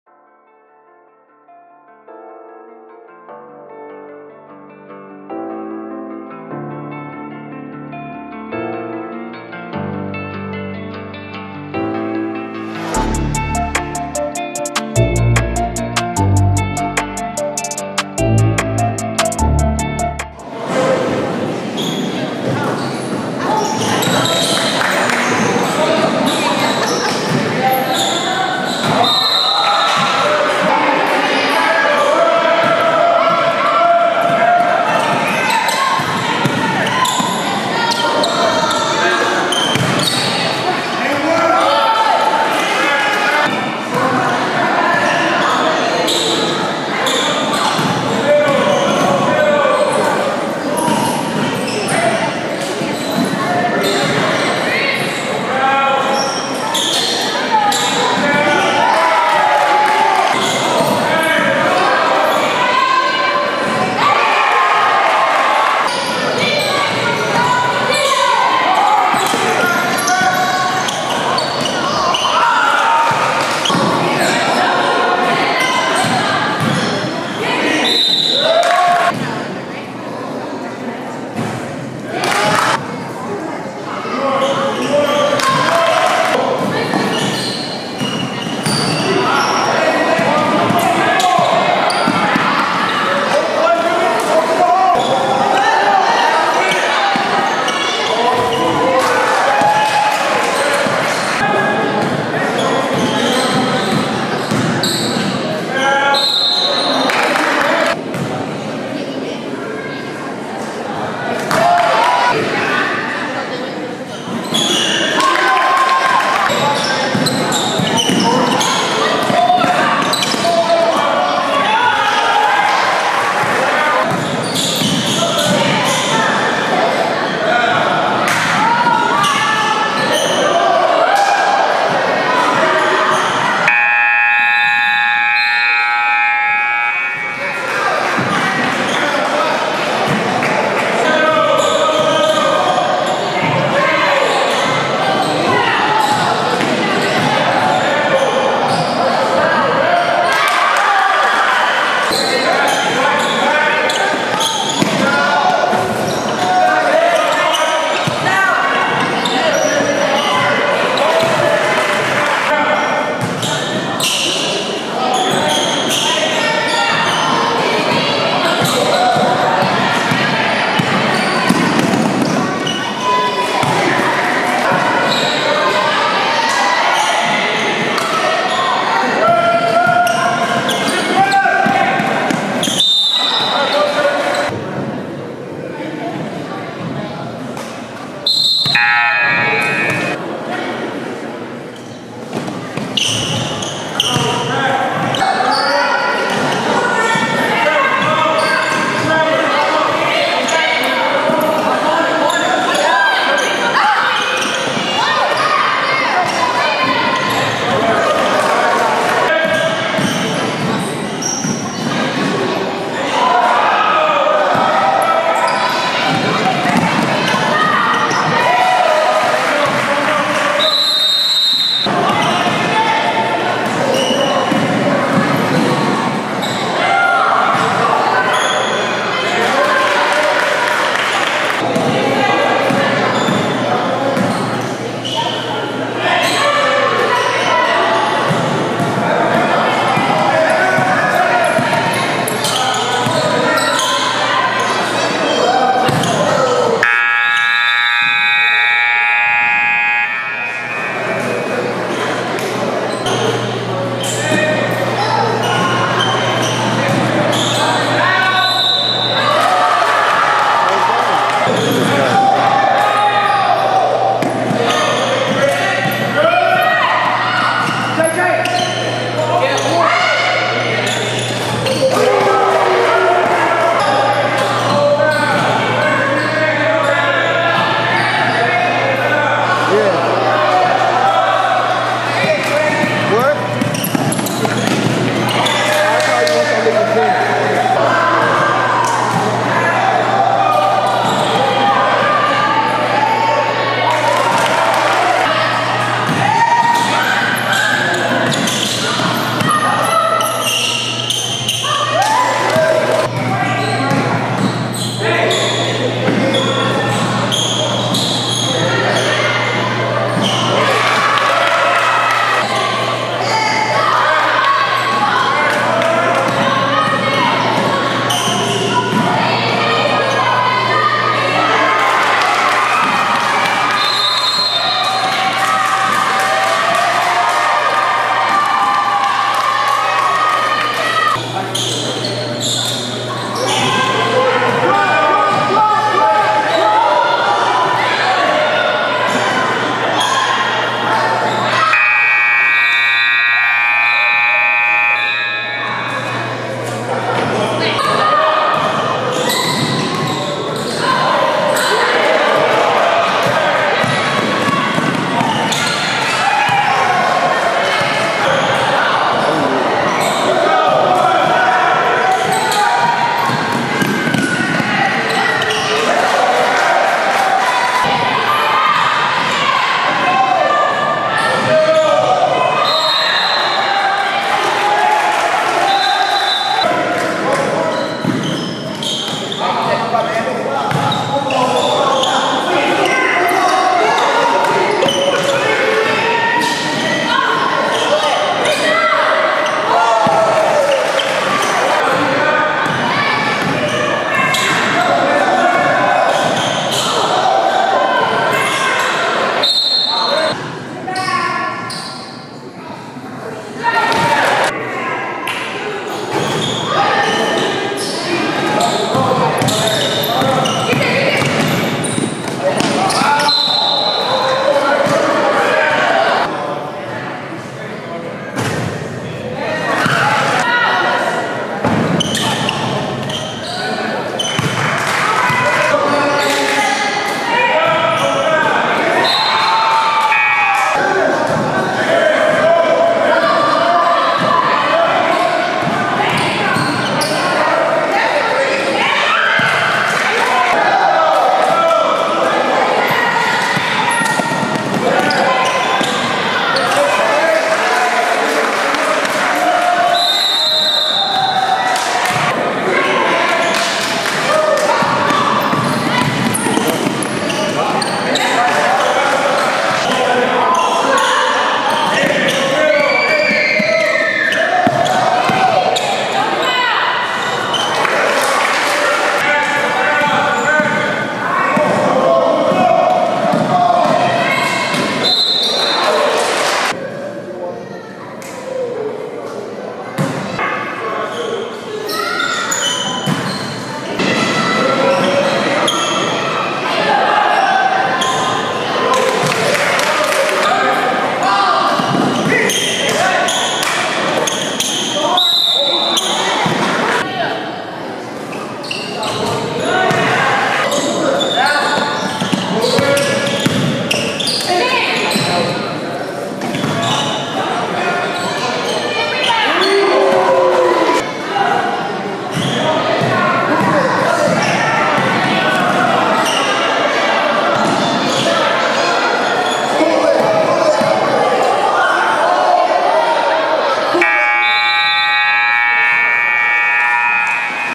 Brookstone vs Mallard Creek: Middle School Basketball Playoff Semi-Final TALENTED YOUNG HOOPERS!!!
Double Dribble Hoops features playoff semi-final basketball between Brookstone and Mallard Creek. Mallard Creek dominated all the way though. Both team has hoopers with a lot of upside. Mallard Creek outscored Brookstone 50-29. Double Dribble Hoopz will continue to showcase top tier talent and games in the Charlotte area.